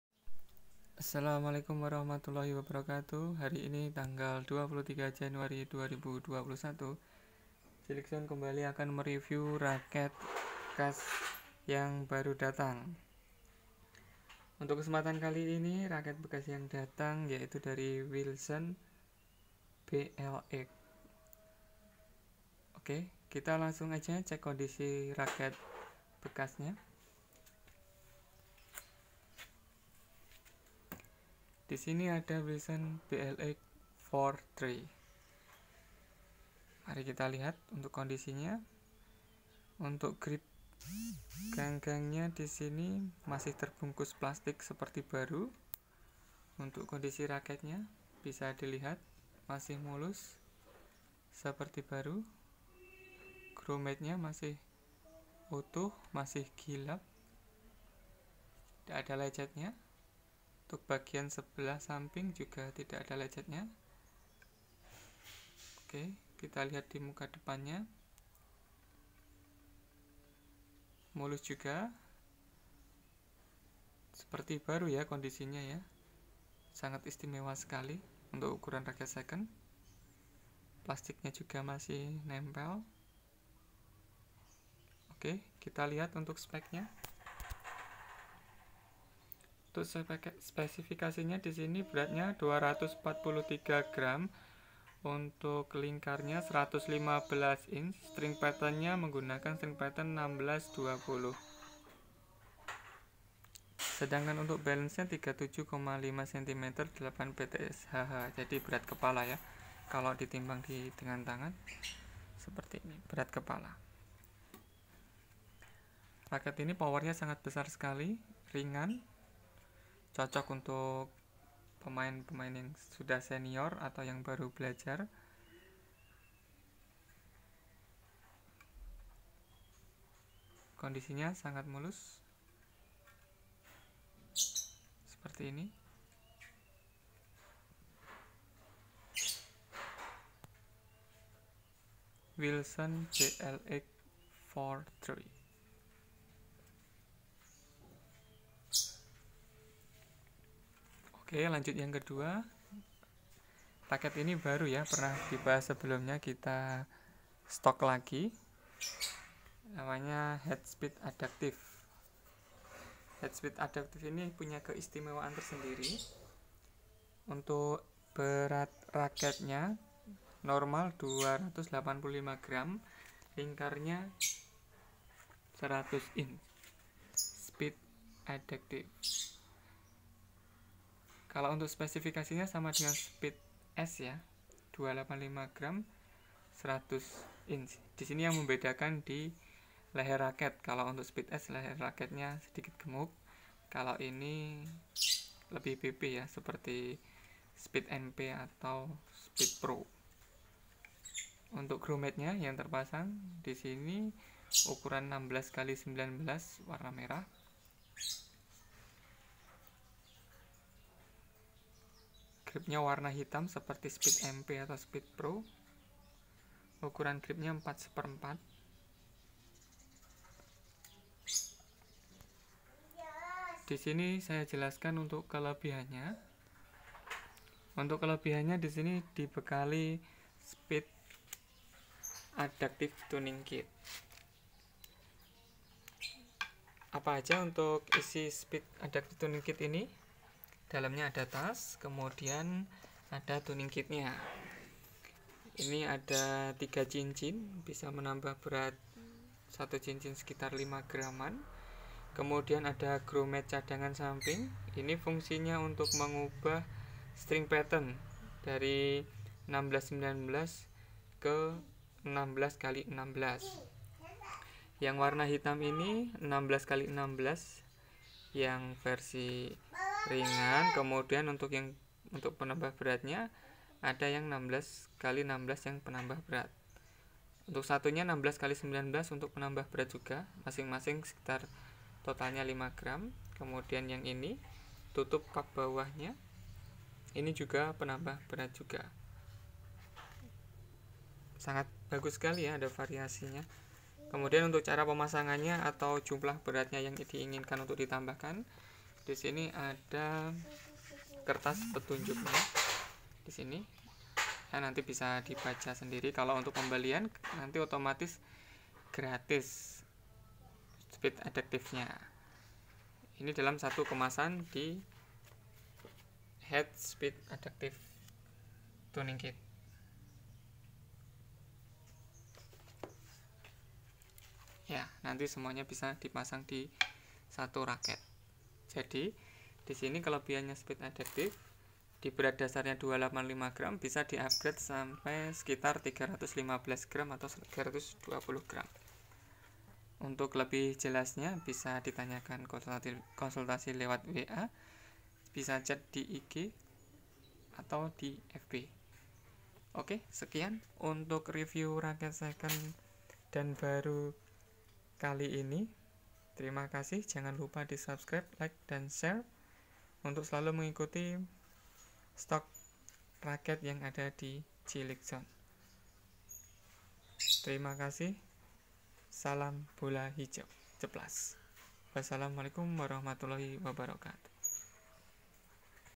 Review WiLson BLX four three dan Speed Adaptiv Raket tenis second
stok tgl 23 jan 2021

1 . wilson blx four three

243 gr
115 inch

2. speed adaptiv

285 gr
100inch
+tuning kit

info pemesanan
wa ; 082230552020